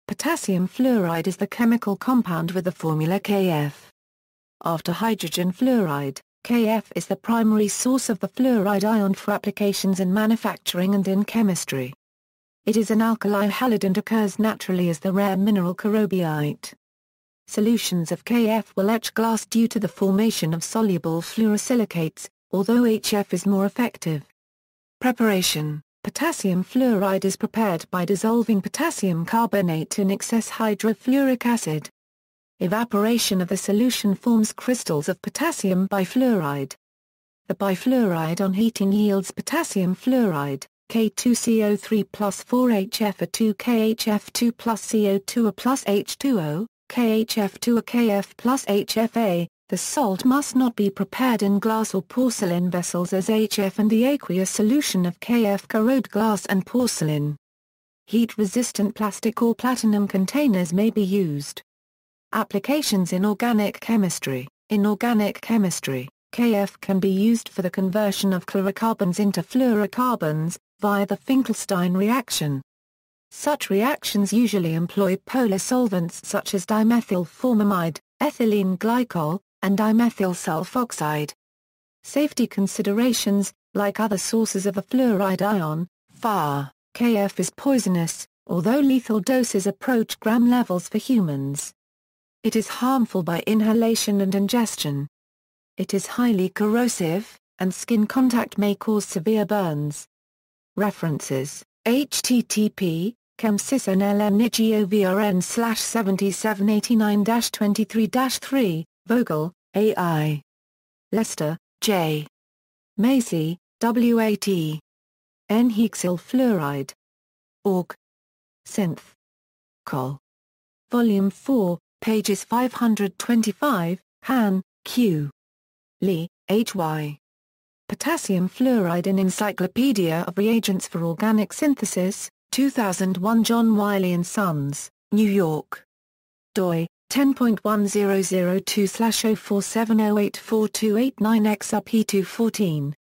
Potassium fluoride
Potassium fluoride is the chemical compound with the formula KF. After hydrogen fluoride, KF is the primary source of the fluoride ion for applications in manufacturing and in chemistry. It is an alkali halide and occurs naturally as the rare mineral carobbiite. Solutions of KF will etch glass due to the formation of soluble fluorosilicates, although HF is more effective.